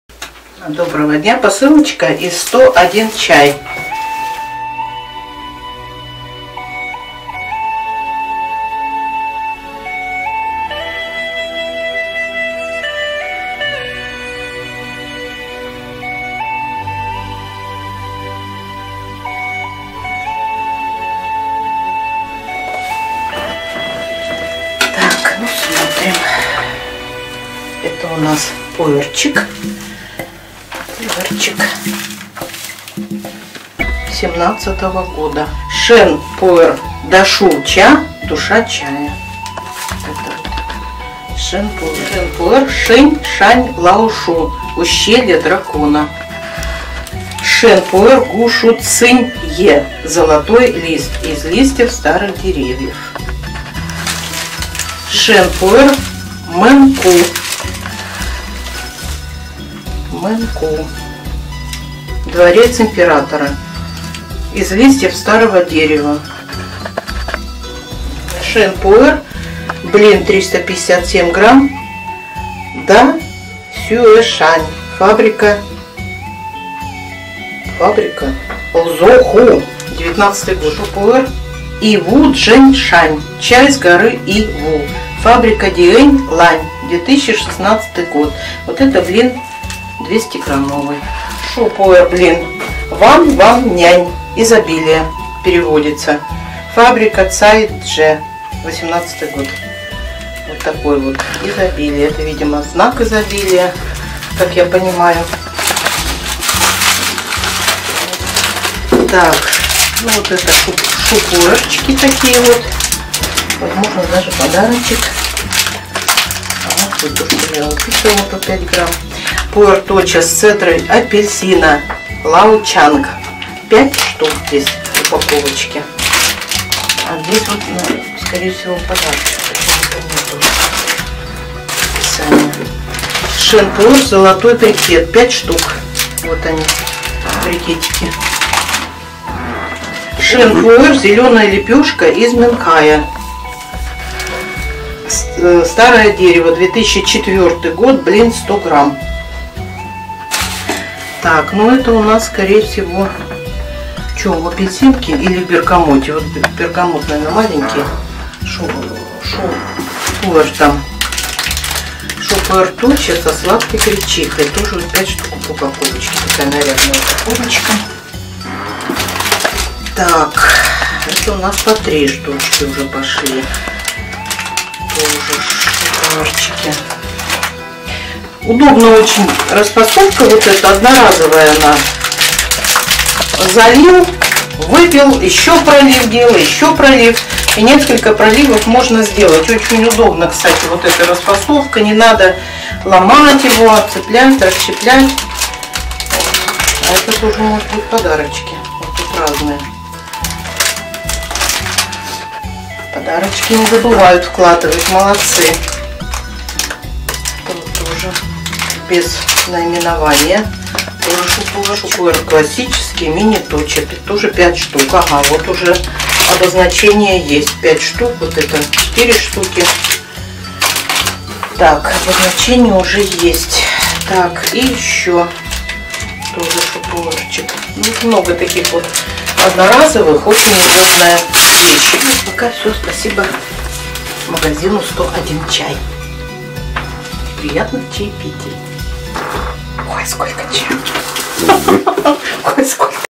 Посылка из магазина "101чай", 2021. / Package from the store "101 tea", 2021.
Видеообзор посылки из интернет-магазина "101чай". /  Video review of the parcel from the online store "101tea".